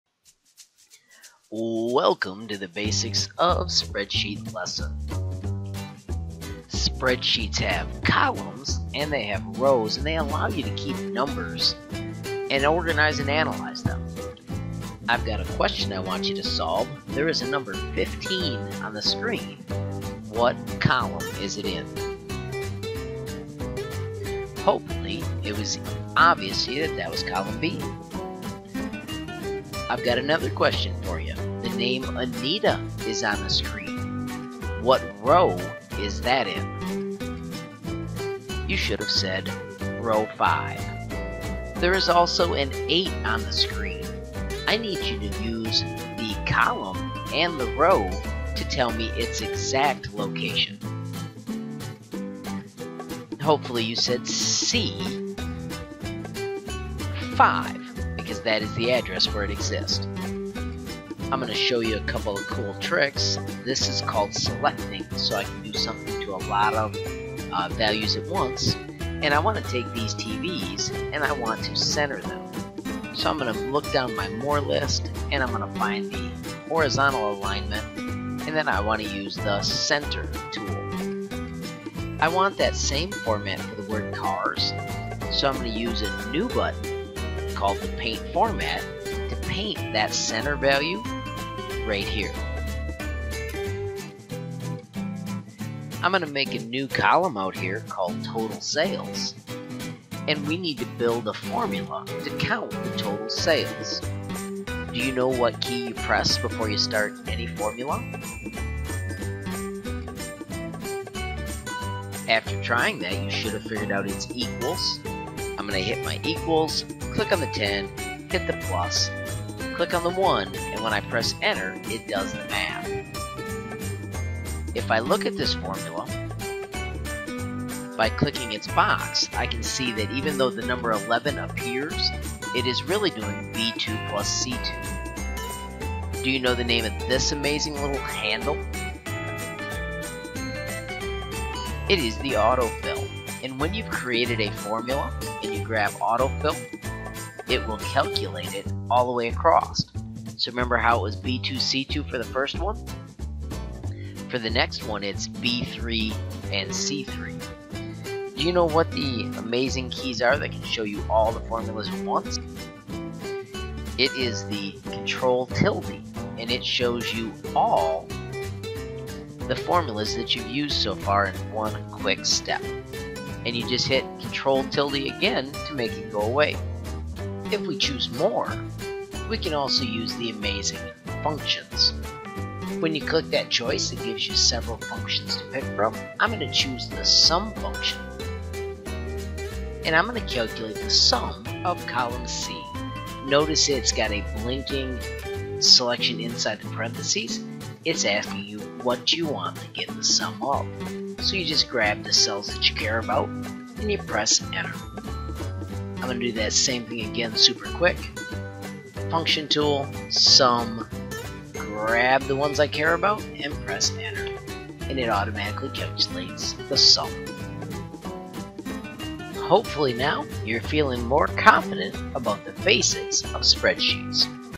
basics of spreadsheets
A super quick explanation of spreadsheets. It has been designed to be incorporated into a EdPuzzle which will be a formative assessment.